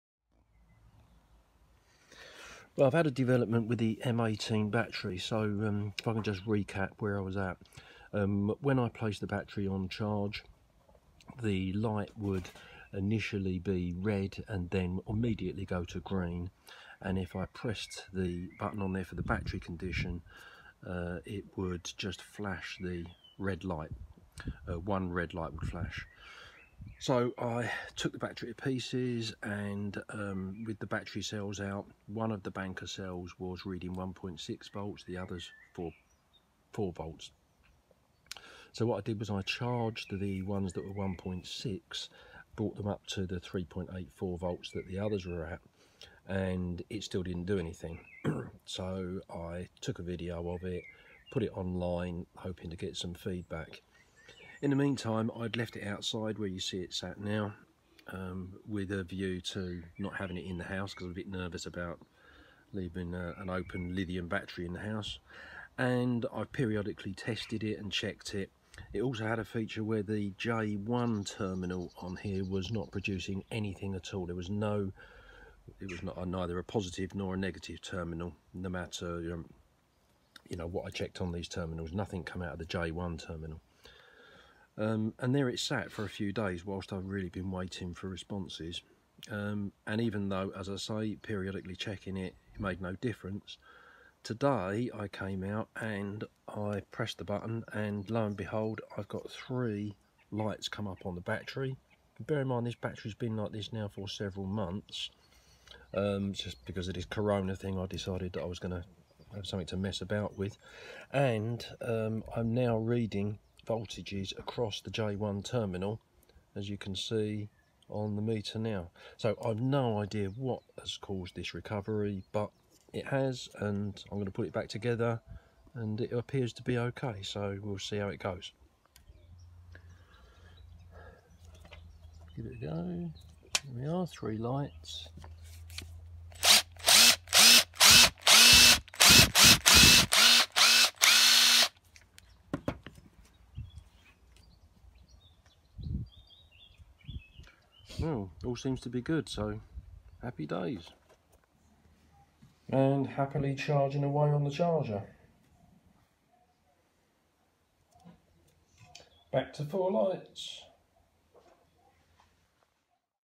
Milwaukee M18 Battery - Teardown & Fixed - Part 2
Milwaukee M18 would not charge or work but on stripdown showed an inbalance of cell charge - Part 1 shows the specific issues, part 2 - this video - shows what happened and the resulting "fix" - if you can call it that...